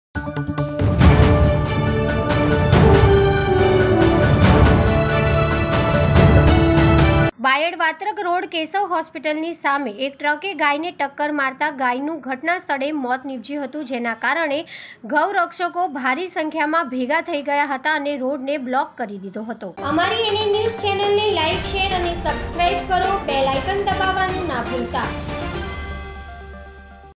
બાયડ વાત્રક રોડ કેશવ હોસ્પિટલ સામે ટ્રક ટક્કર મારા ગાયનું મોત
બાયડ વાત્રક રોડ કેશવ હોસ્પિટલ સામે  ટ્રક ટક્કર મારા ગાયનું મોત ગૌરક્ષક વાળા રોડ બ્લોક પ્રદર્શન જાહેર કર્યું મોટી સંખ્યામાં .બાયાડ.એન એન નુએસ
પાર્થસોની અરવલ્લી